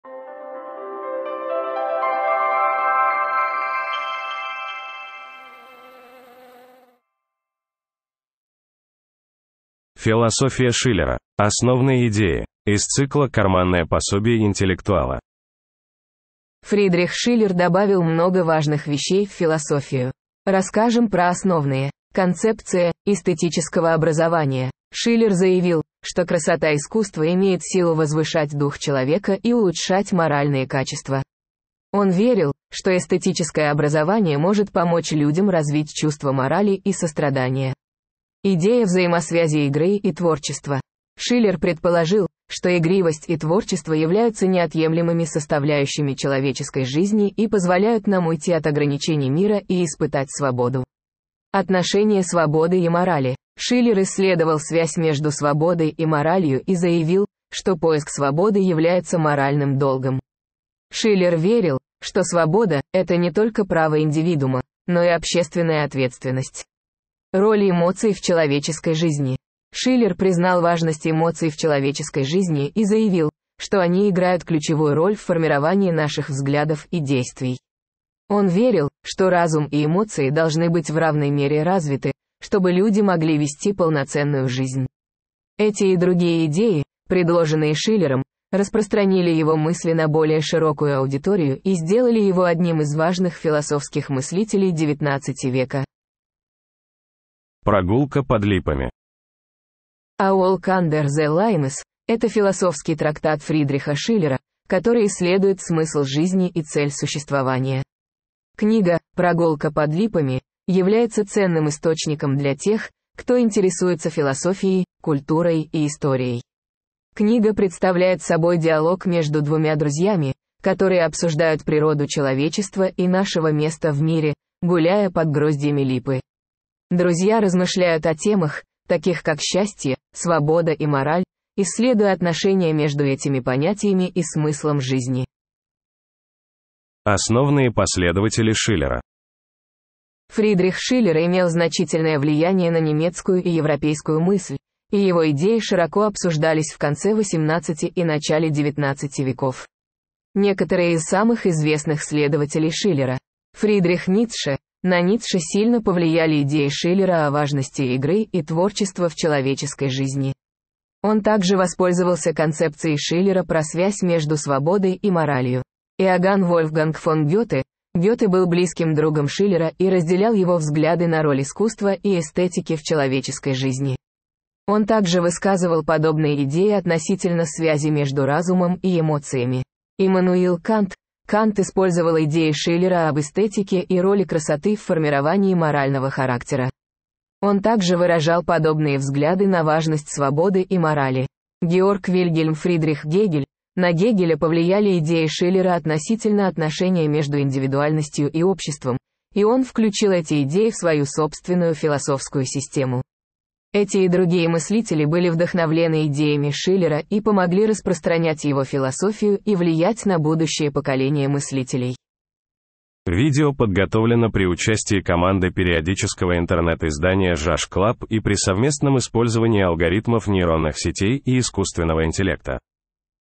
Философия Шиллера: основные идеи
Фридрих Шиллер имел значительное влияние на немецкую и европейскую мысль, и его идеи широко обсуждались в конце 18 и начале 19 веков.